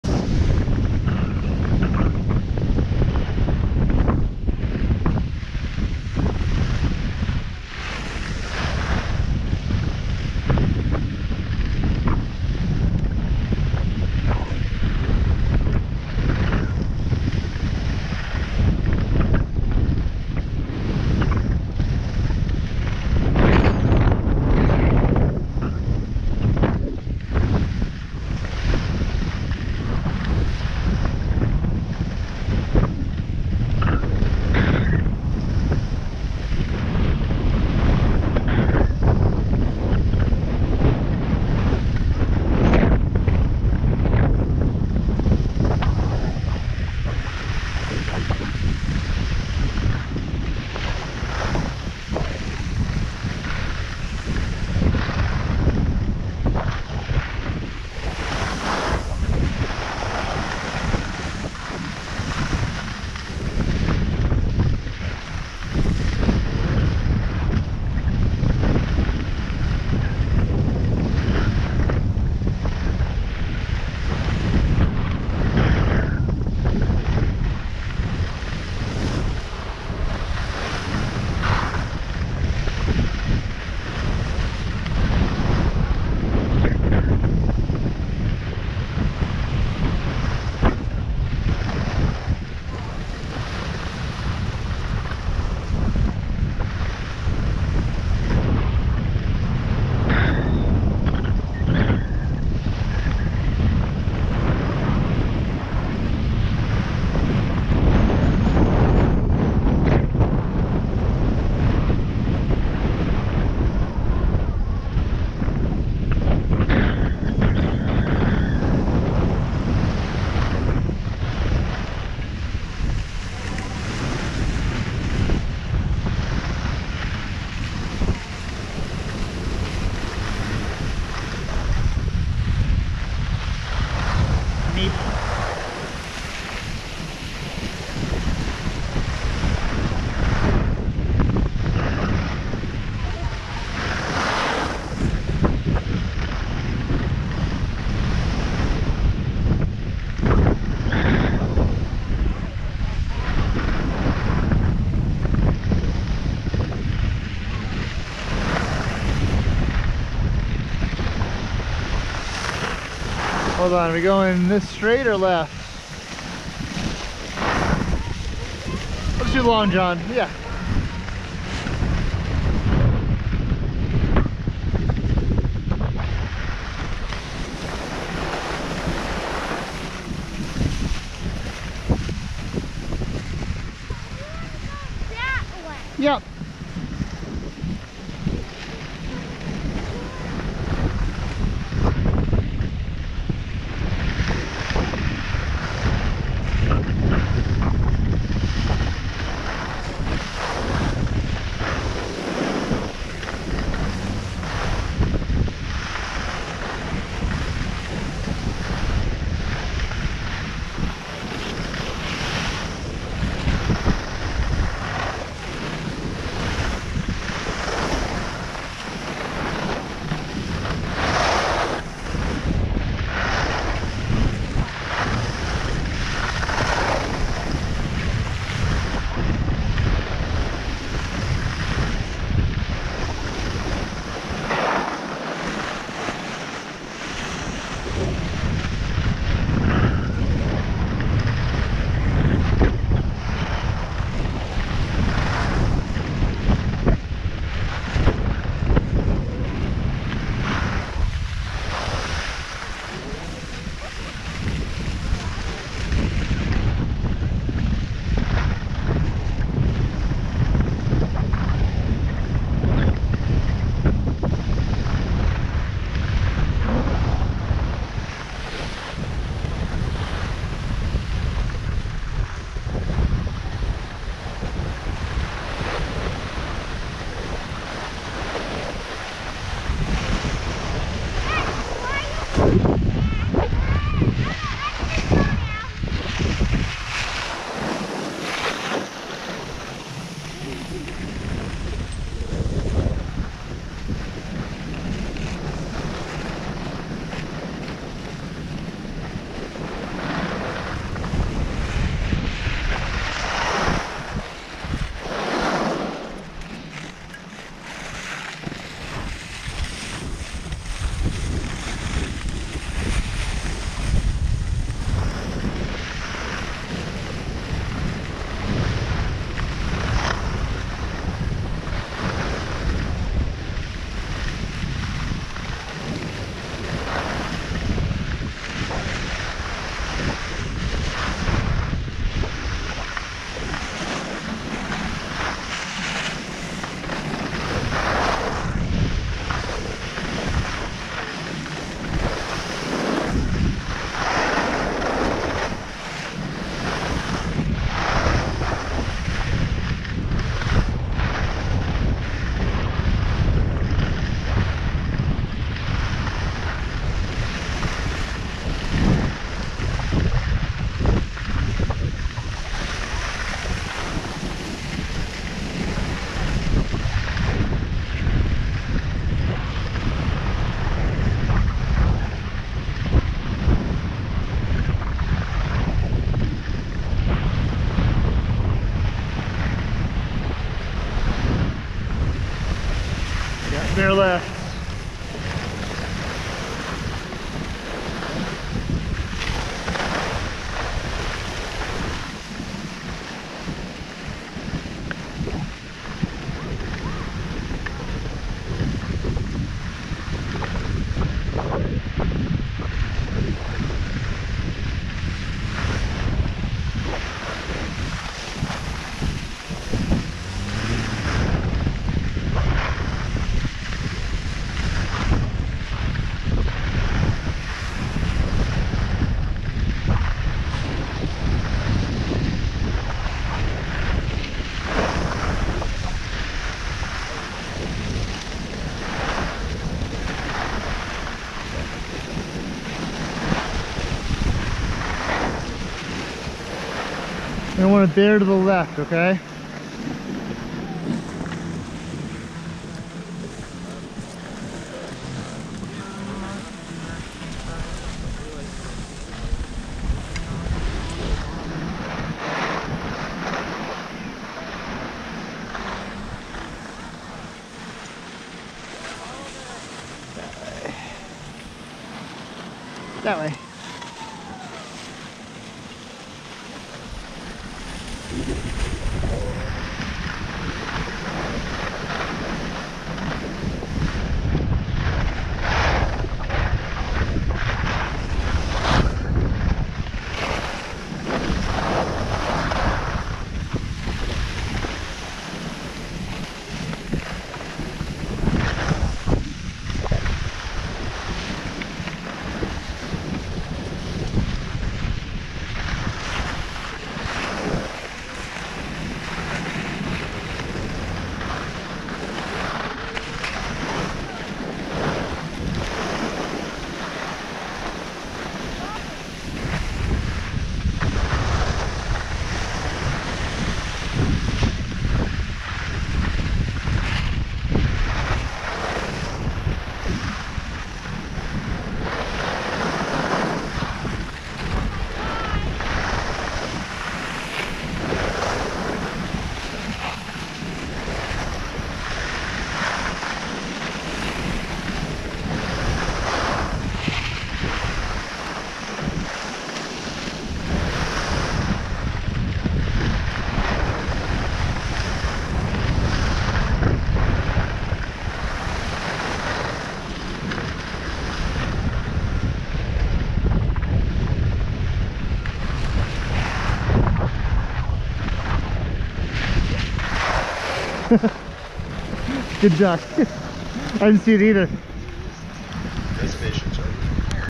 AJ Run - 2/28/2021 - Mount Snow, Long John
Not much to this one, just a simple top-to-bottom meandering run down Long John.